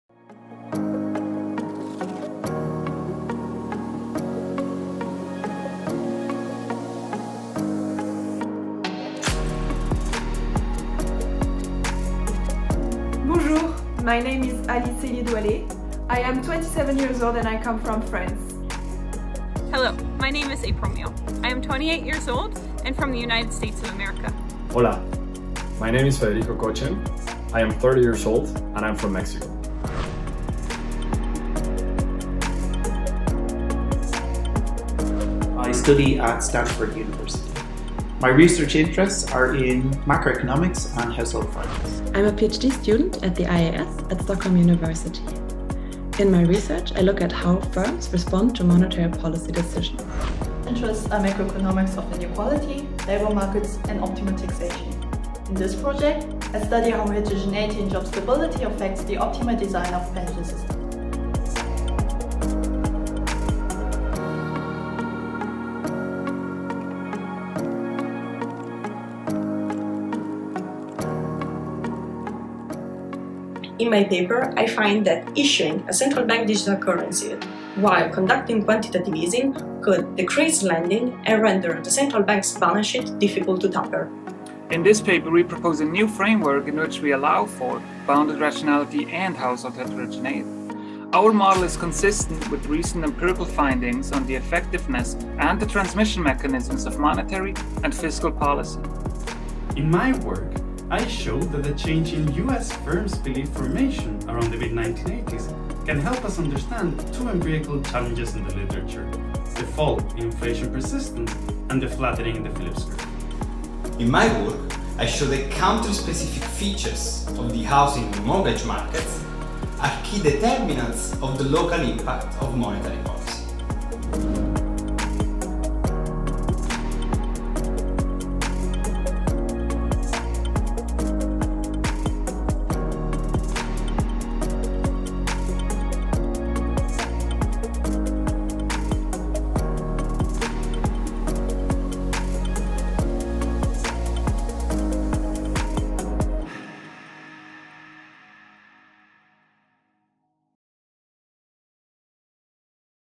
Young Economist Prize 2022
Young economists can play an important role in shaping the future of Europe. Every year we recognise this through the Young Economist Prize, a research competition that offers talented students the chance to share their fresh perspectives on today’s challenges.

Finalists are invited to the annual ECB Forum on Central Banking, and the overall winner is awarded €10,000.